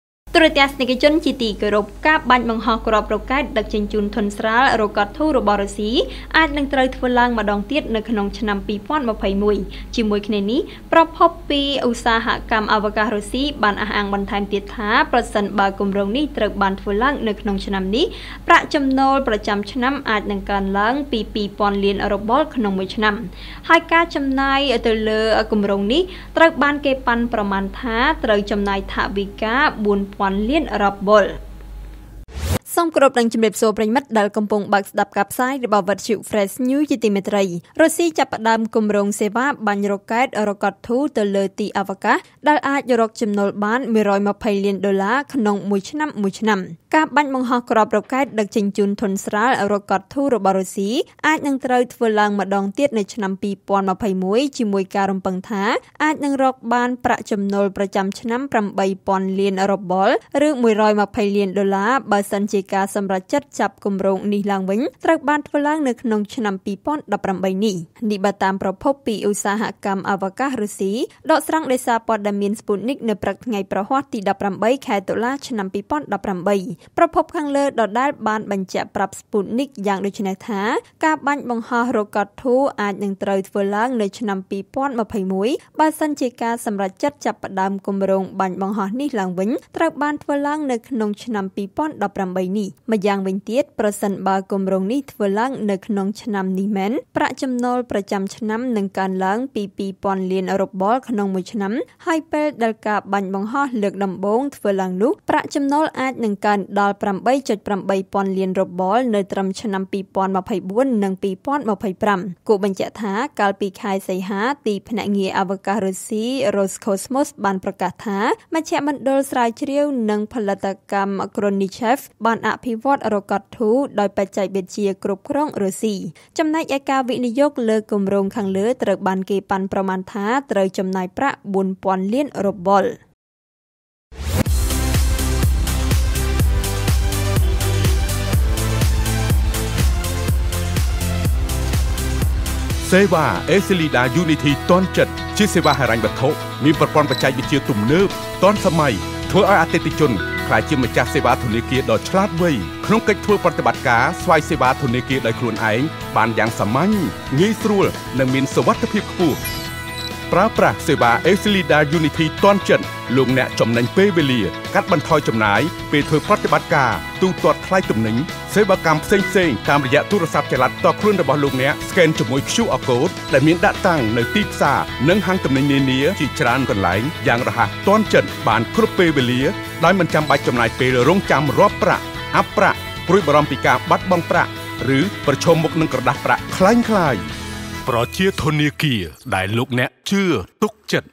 ​រុស្ស៊ីចាប់ផ្តើម​គំរោងសេវាបាញ់រ៉ុក្កែត​ Rokot-2 ទៅទីអវកាស ដែលអាច​រកចំណូល...
​រុស្ស៊ីចាប់ផ្តើម​គំរោងសេវាបាញ់រ៉ុក្កែត​ Rokot-2 ទៅទីអវកាស ដែលអាច​រកចំណូលបាន​១២០លានដុល្លារ​ក្នុងមួយឆ្នាំៗ (រាយការណ៍ព័ត៌មានដោយកញ្ញា សំ ស្រីនីត, ទូរទស្សន៍ Fresh News )

ទូរទស្សន៍ Fresh News កំពុងផ្សាយព័ត៌មាន២៤ម៉ោង, ផ្សាយតាមរយៈ Smart TV, App, Website, Radio, Facebook និង Youtube